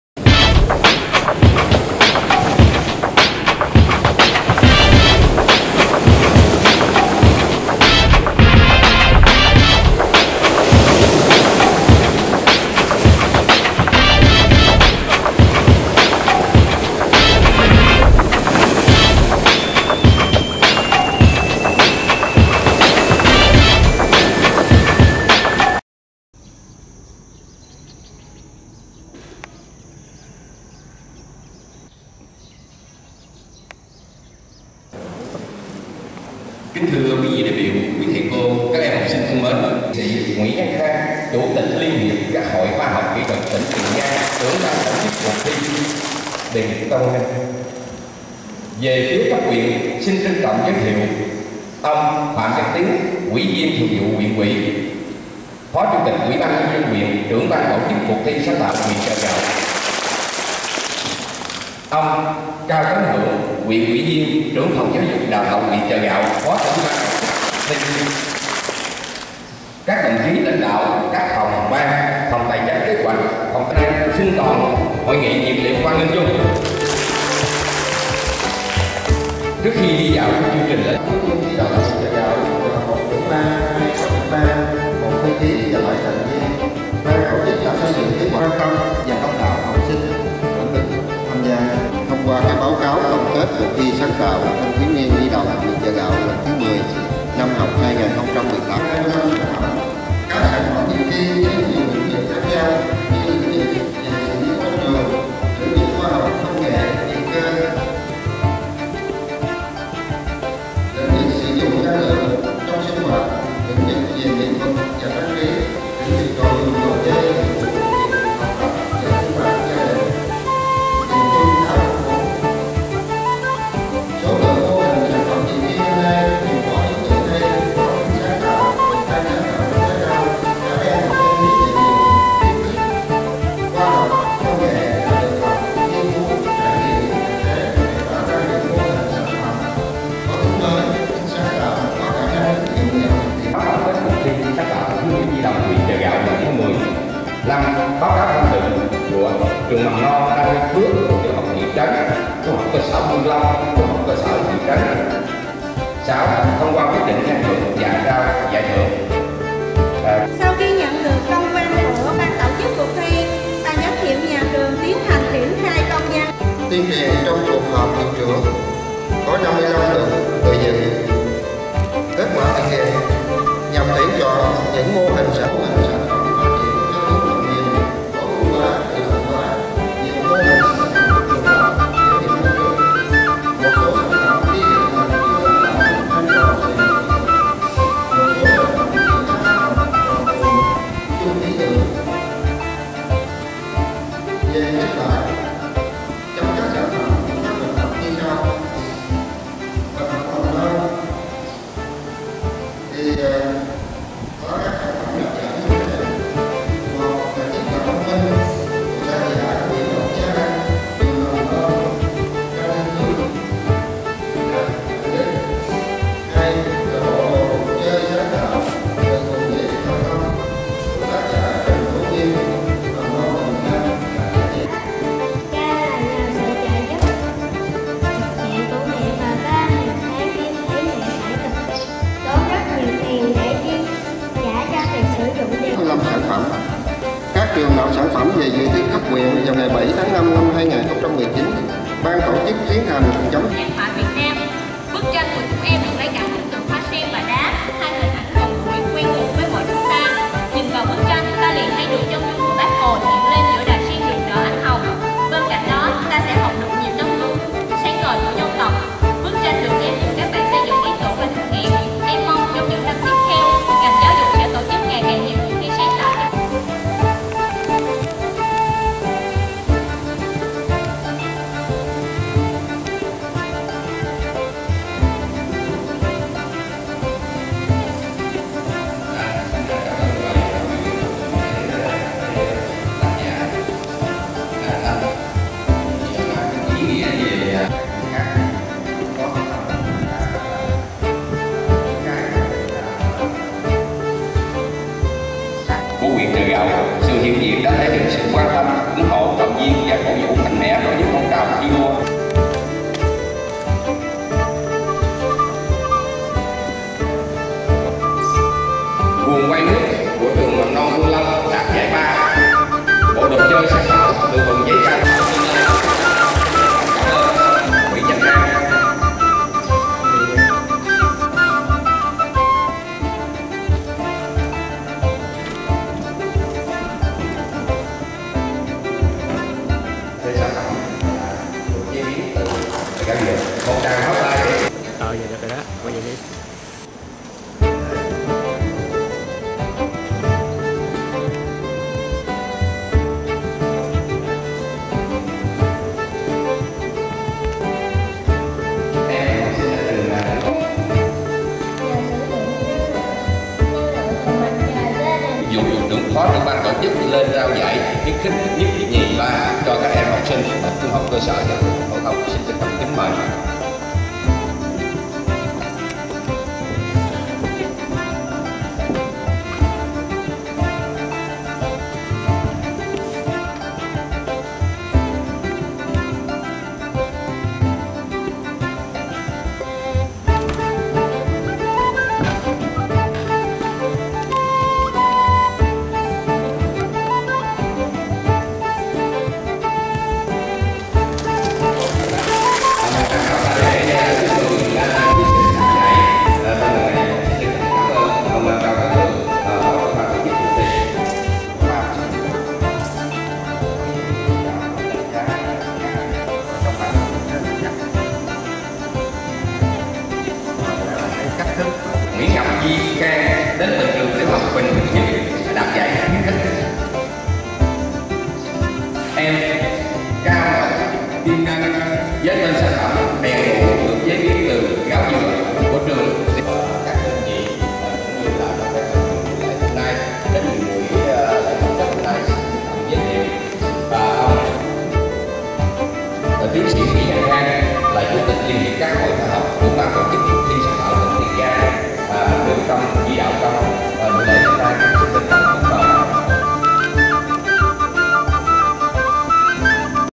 ĐÂY TIỀN GIANG - CHỢ GẠO TRAO THƯỞNG CUỘC THI SÁNG TẠO TTN VÀ NĐ 2019
Vicky media 23/5/2019
Sáng ngày 23/5, tại Trường THCS Thị trấn Chợ Gạo, BTC Cuộc thi Sáng tạo Thanh thiếu niên và Nhi đồng tỉnh Tiền Giang lần thứ XII, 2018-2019 tổ chức tổng kết trao thưởng cho các tác giả đạt giải.
Lần này huyện Chợ Gạo có nhiều tác phẩm dự thi so lần trước với nhiều sản phẩm, mô hình phong phú. BTC chấm 151 tác phẩm dự thi cấp huyện và trao thưởng cho 19 tác phẩm, đồng thời đưa các tác phẩm đạt giải cao dự Cuộc thi cấp tỉnh .